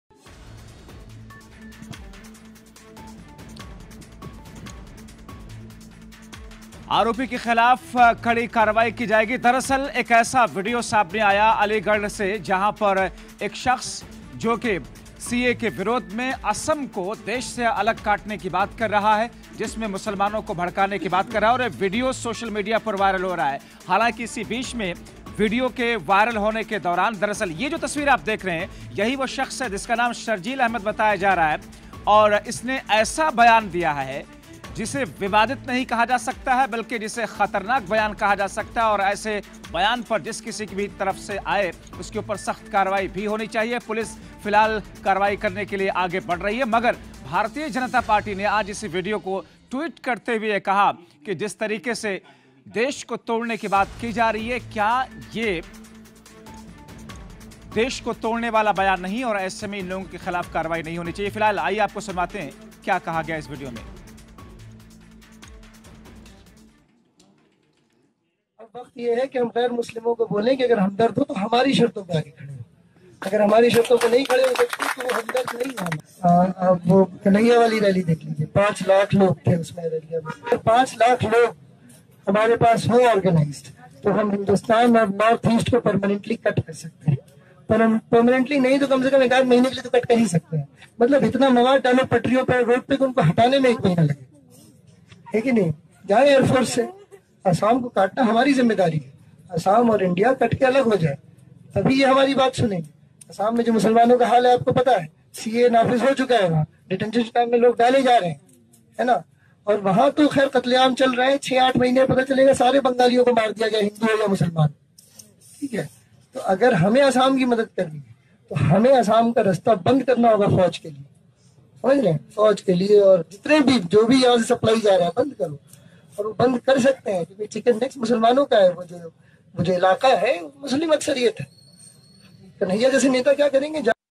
AMU में देश विरोधी बयानबाजी, SSP ने दिए केस दर्ज करने के आदेश | R9TV
AMU में जो देश विरोधी बयानबाजी का वीडियो वायरल हुआ था… जिसके बाद SSP ने केस दर्ज करने के आदेश दिए हैं...